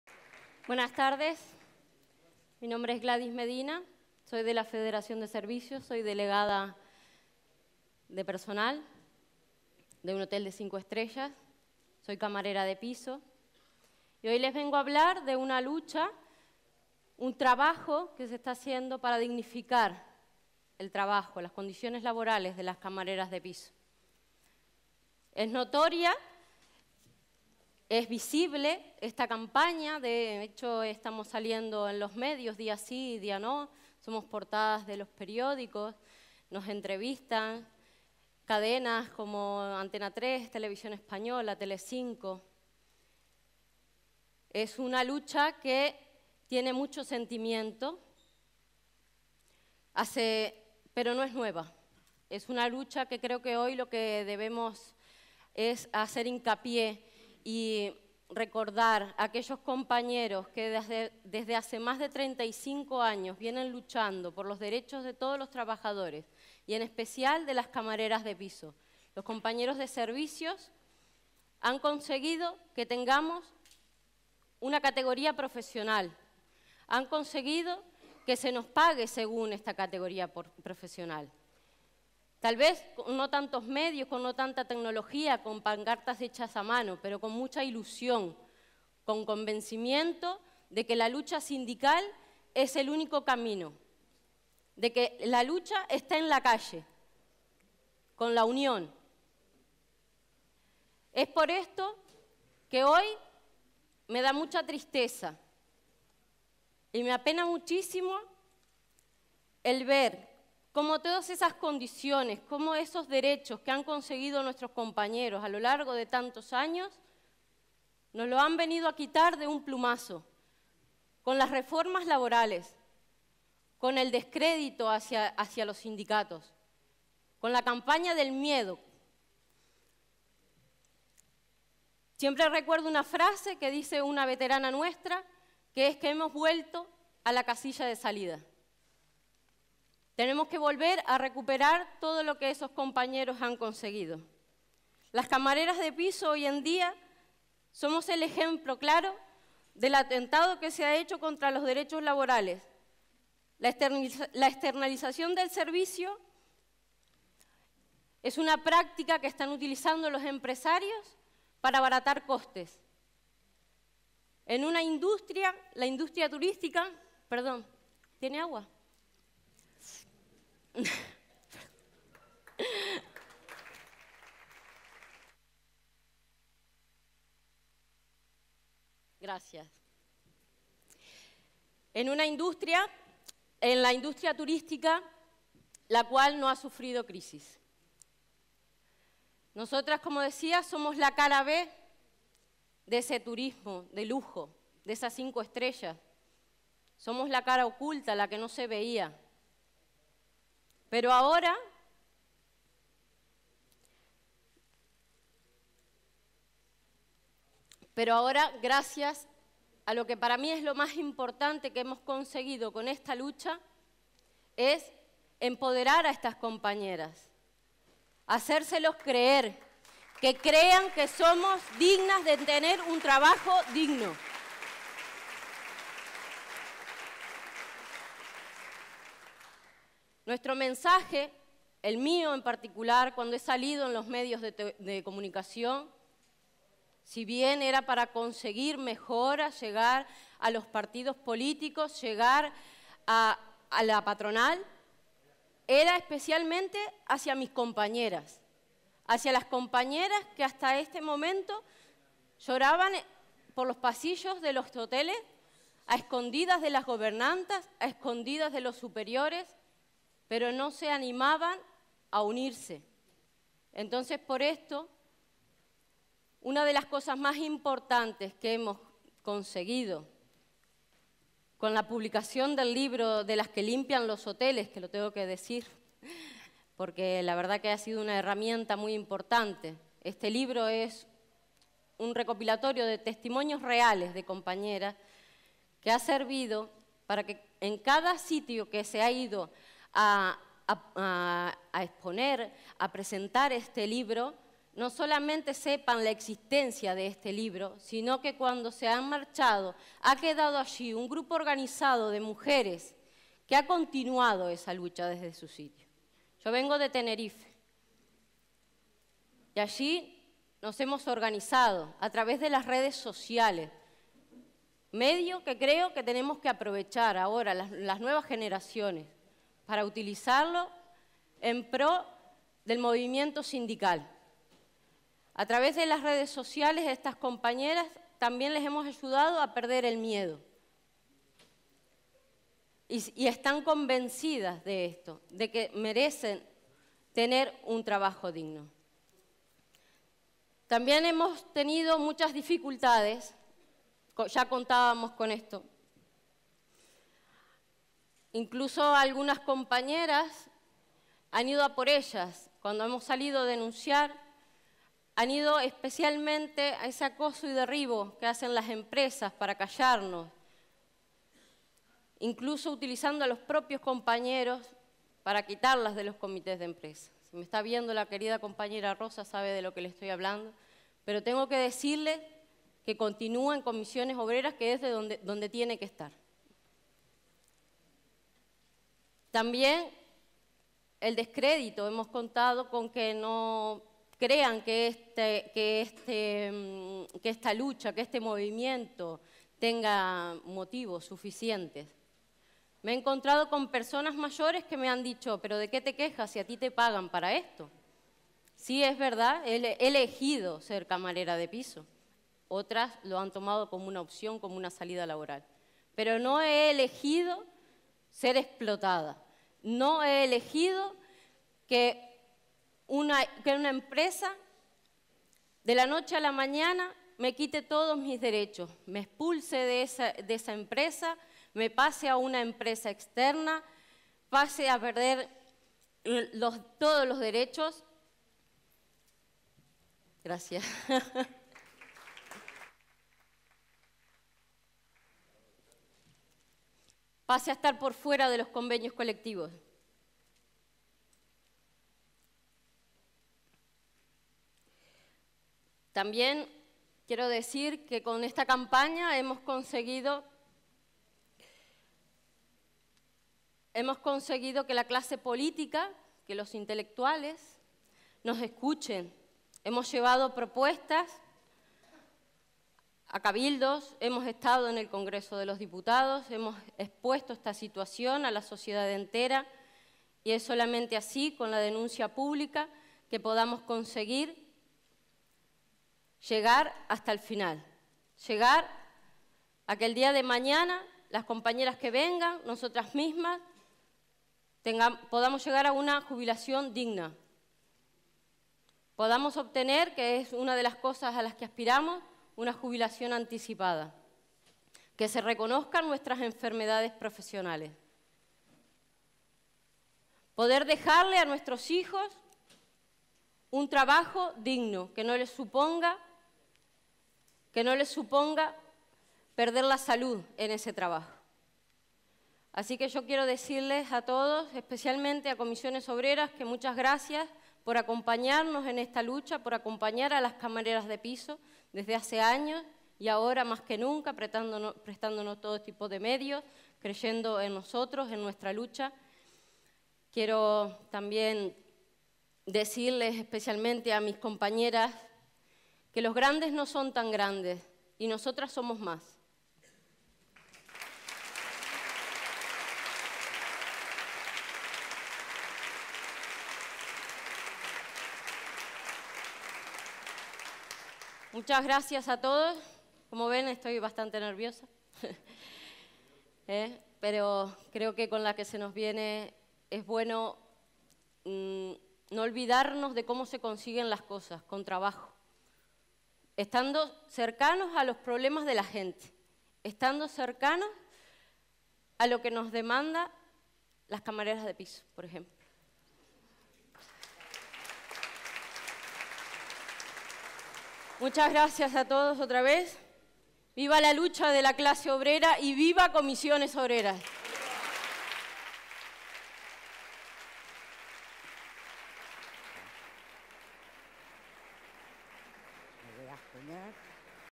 Intervención Gladys Medina 40 aniversario de la Asamblea de Barcelona
40 aniversario de la Asamblea de Barcelona  acto en la antigua Fabra i Coats. Intervención de Gladys Medina, delegada de Servicios CCOO Canarias, sobre la precariedad que sufren las camareras de piso tras la aplicación la Reforma Laboral.